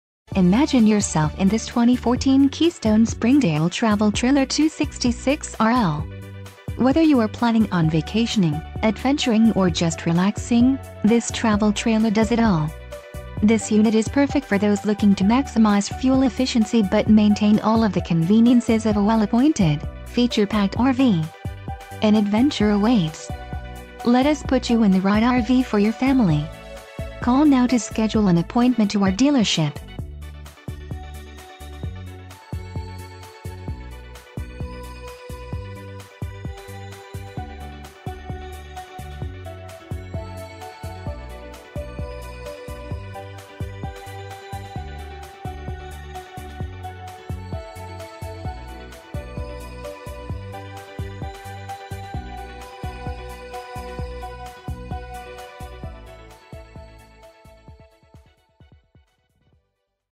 2014 Keystone Springdale Travel Trailer - Haylett Auto and
Options and Upgrades on this 2014 Springdale 266RLTravel Trailer by Keystone RV include:\r\n* Springdale Adventure Package! This limited-time series of upgrades includes a larger 7 cubic foot refrigerator and freezer with one-hand open operation, multimedia power center to charge your devices, DVD/Bluetooth/CD Stereo, Black Tank flush system, Outside TV Hookup with Bracket to take your indoor TV outside, LED Awning light strip, Electrolux Vacuum Cleaner, Full Body Graphics, and a friction hinged entry door so the wind doesn\'t slam your door against the side of your camper!\r\n* We\'ve enclosed the underbelly of this Springdale RV to protect your trailer during transit and extend your camping season!\r\n* An LCD Television with inside and outside camper mounts has been included so you never have to miss a game or race!\r\n* Radial tires have been installed to provide maximum reliability while towing your travel trailer around our great nation!\r\n* The exterior shower we\'ve added to t...read more